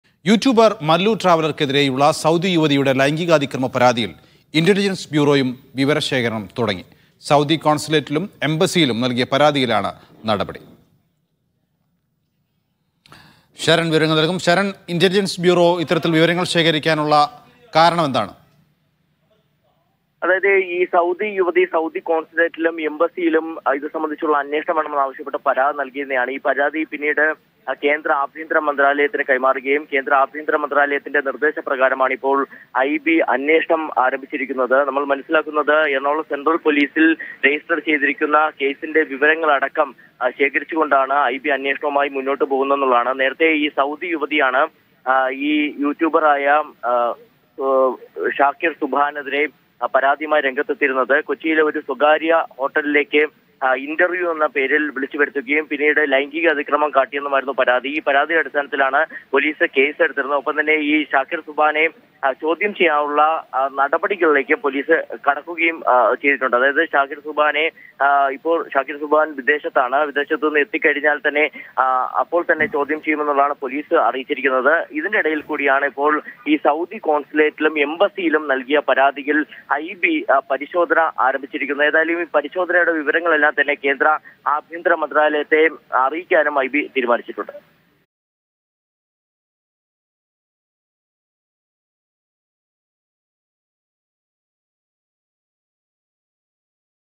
മല്ലു ട്രാവലർക്കെതിരെയുള്ള ലൈംഗികാതിക്രമ പരാതിയിൽ IB അന്വേഷണം ആരംഭിച്ചു
മല്ലു ട്രാവലർക്കെതിരെയുള്ള ലൈംഗികാതിക്രമ പരാതിയിൽ ഇന്റലിജൻസ് ബ്യൂറോയും അന്വേഷണം ആരംഭിച്ചു | Mallu Traveler | Shakeer Suban |